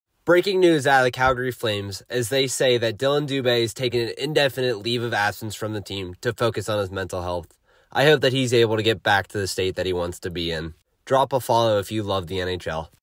Dillon Dube is Stepping Away from the Calgary Flames!
Best wishes to Dillon Dube